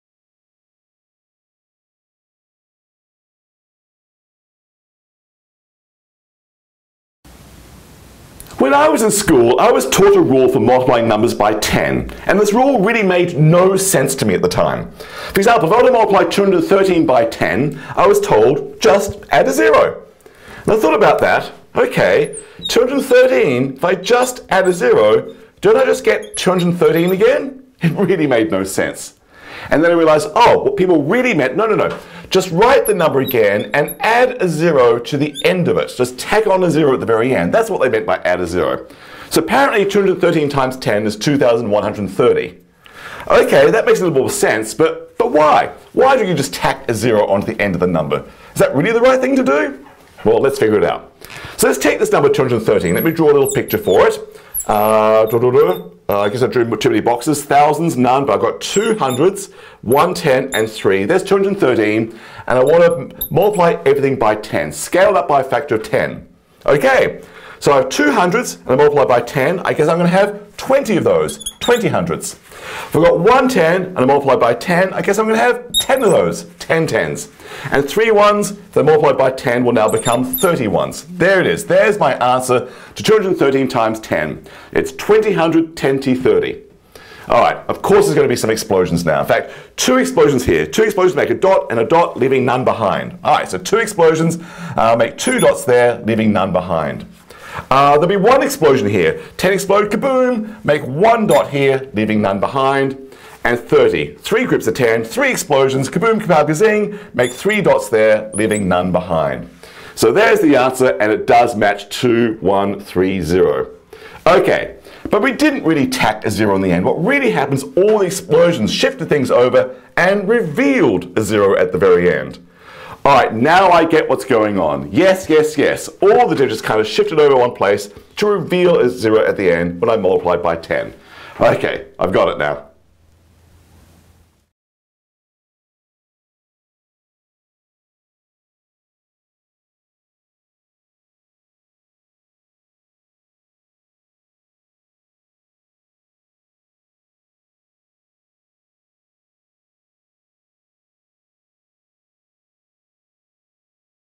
11. "To Multiply by Ten, Add a Zero." HUH? (Exploding Dots)
This is video number 11 in the Exploding Dots story.

The written guide to go with these videos and access to the full playlist is here:

Also see ** ** for the global math phenomenon afoot.